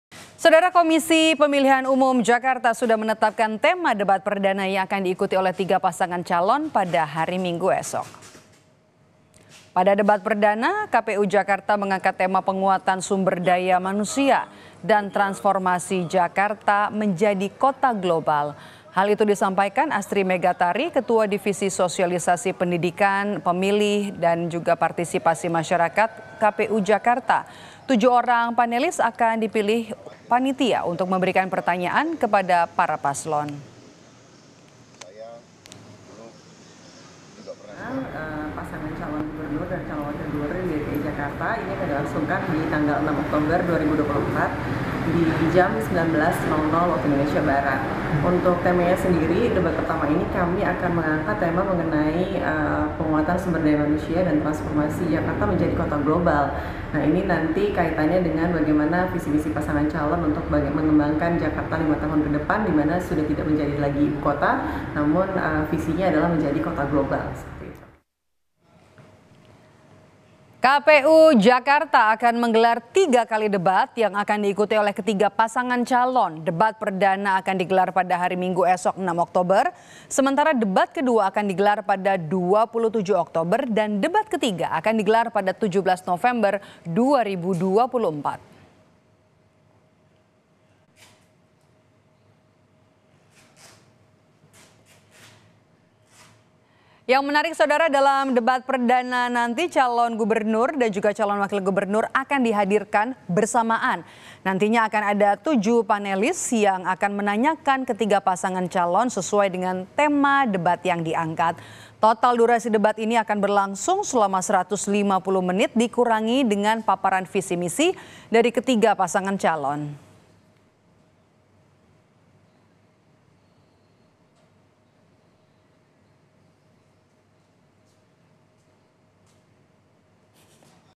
KPU Jakarta Usung Tema Debat Perdana Pilkada Jakarta, Adu Gagasan dalam Transformasi Kota
KOMPAS.TV - Komisi Pemilihan Umum (KPU) Jakarta sudah menetapkan tema debat perdana yang akan diikuti tiga pasangan calon pada Minggu (6/10) mendatang.

Pada debat perdana ini, KPU Jakarta mengangkat tema "Penguatan Sumber Daya Manusia dan Transformasi Jakarta Menjadi Kota Global."

Hal itu disampaikan oleh Astri Megatari, Ketua Divisi Sosialisasi, Pendidikan Pemilih, dan Partisipasi Masyarakat KPU Jakarta.

Tujuh orang panelis akan dipilih panitia untuk memberikan pertanyaan kepada para pasangan calon.